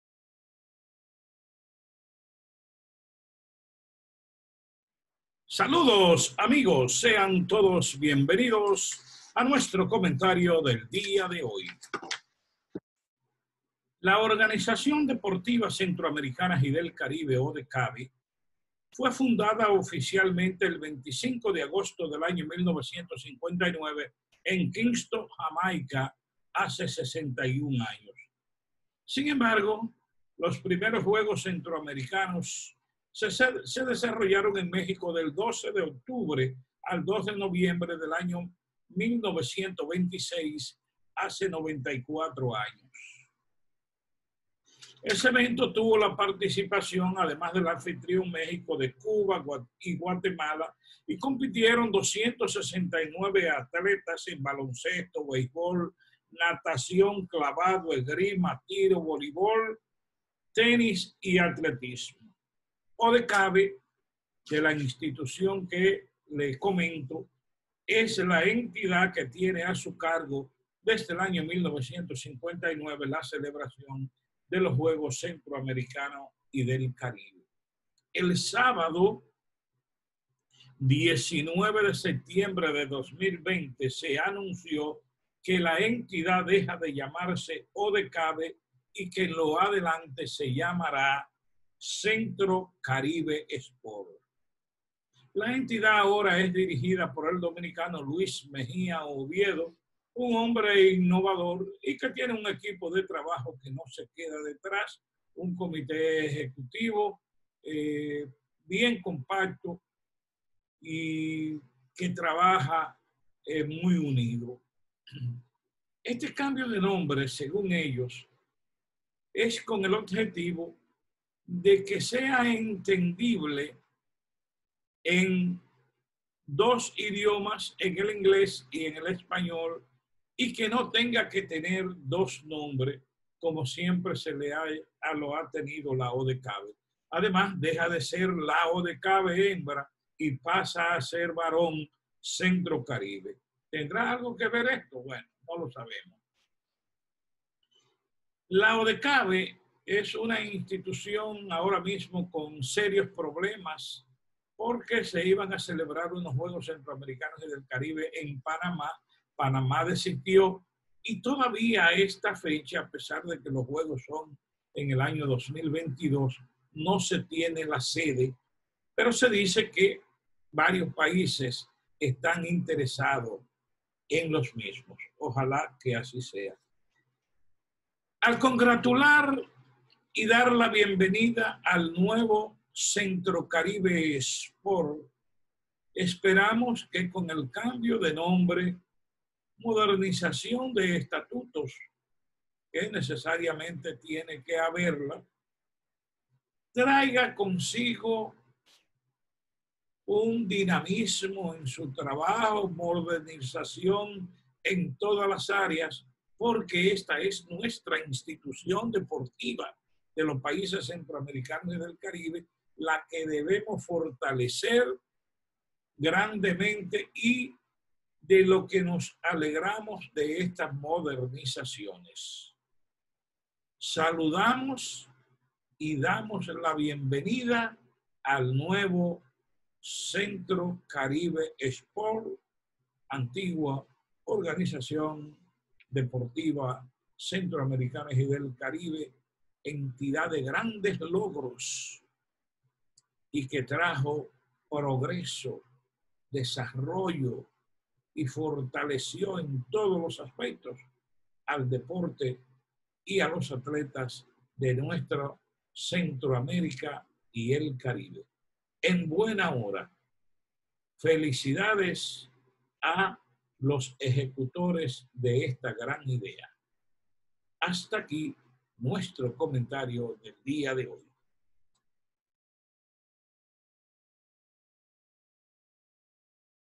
Bienvenido Centro Caribe Sports
El sábado dieron a conocer que la Organización Deportiva Centroamericana y del Caribe (ODECABE) aprobó en Asamblea General Extraordinaria, ni más, ni menos que cambiar su nombre.